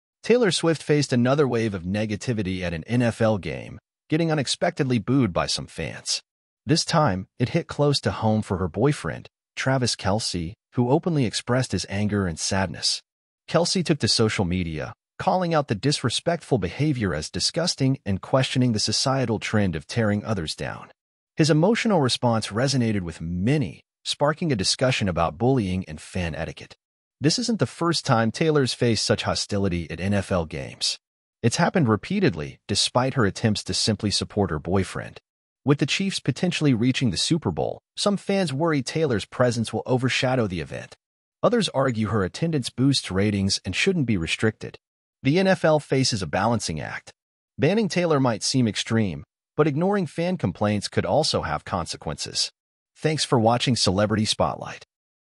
OMG!Taylor Swift Booed, Travis Kelce Left in Tears
Watch the shocking moment when Taylor Swift was booed and NFL star Travis Kelce left in tears. Find out what happened and why in this must-see video. Don't miss the drama and controversy surrounding these popular celebrities!